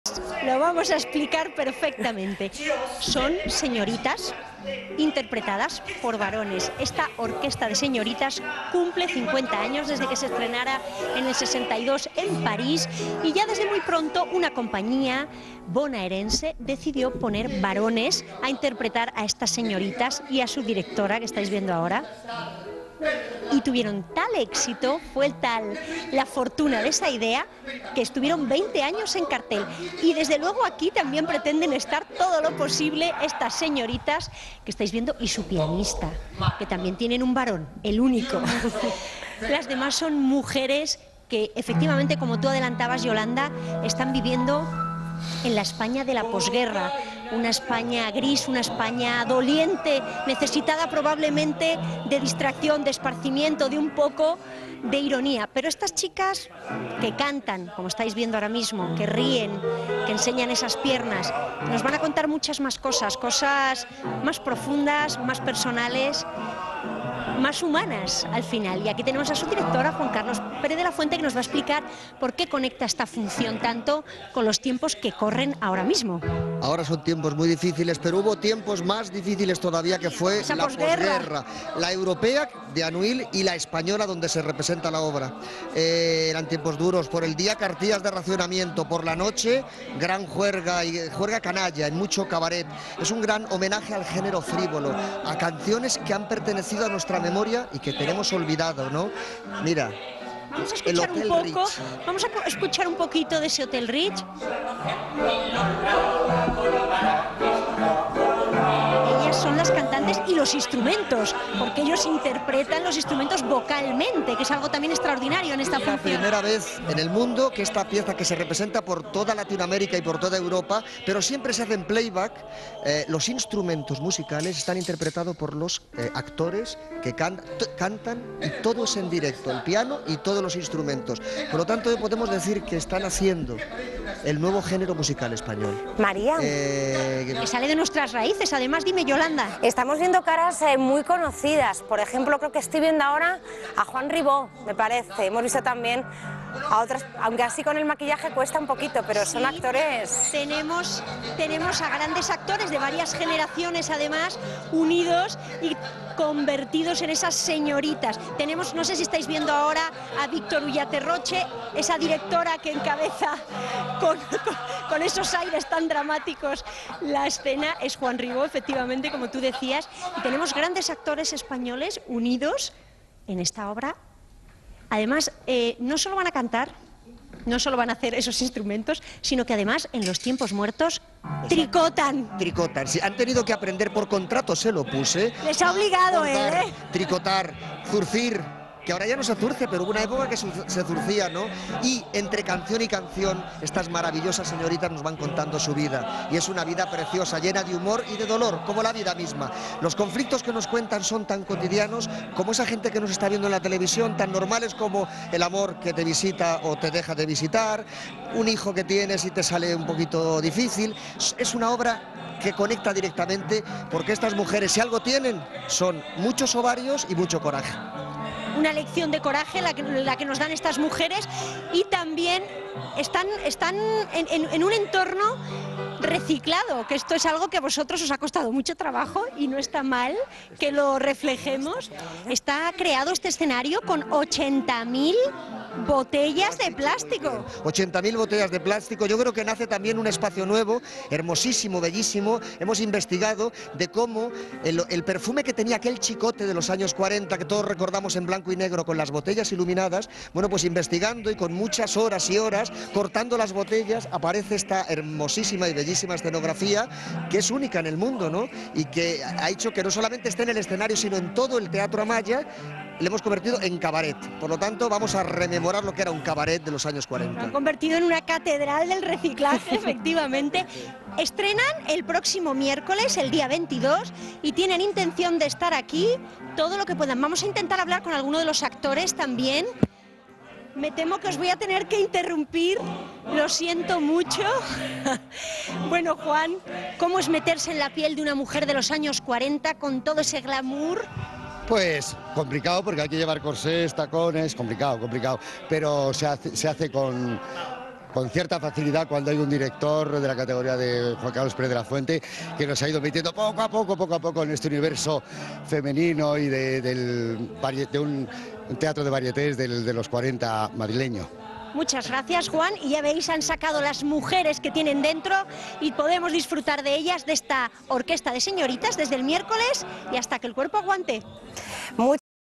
Señoritas de pelo en pecho en el teatro Amaya de Madrid
Llega a Madrid una orquesta muy especial, compuesta por señoritas de pelo en pecho como Juan Ribó, Victor Ullate Jr, Juan Carlos Naya o Emilio Gavira, entre otros. La obra retrata los espectáculos frívolos de la posguerra, un cabaret alegre que también esconde miserias y conflictos atemporales, cantados y contados por esta 'Orquesta de señoritas' del Teatro Amaya.